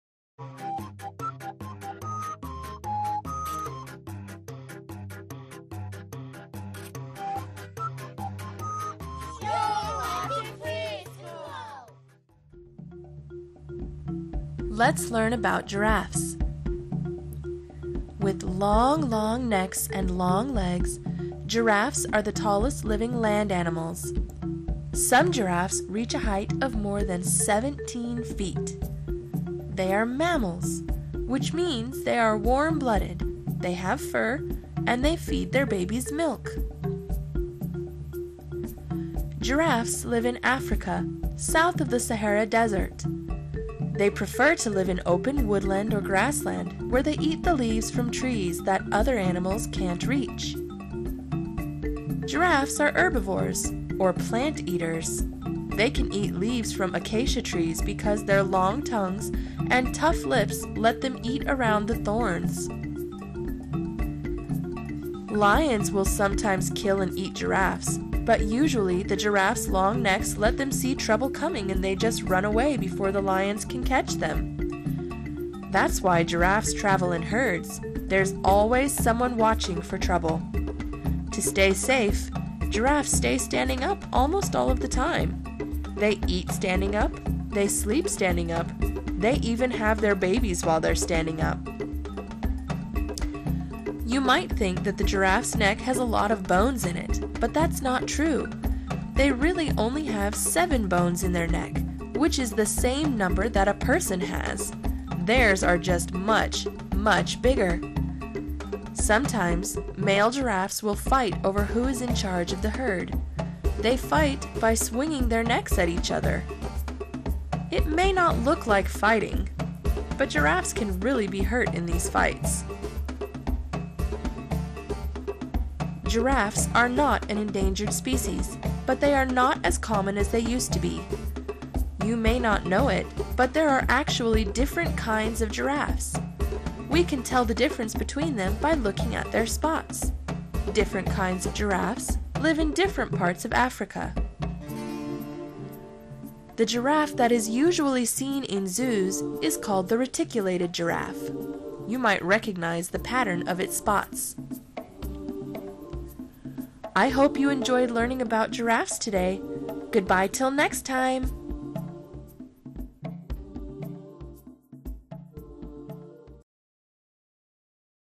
Giraffes for Kids: Learn about Giraffes - FreeSchool
Let's take a look at giraffes and learn a little more about this familiar yet exotic animal. Have you ever wondered, how tall are giraffes? Or, what do giraffes eat? Learn those and more giraffe facts in this fun video! FreeSchool is great for kids!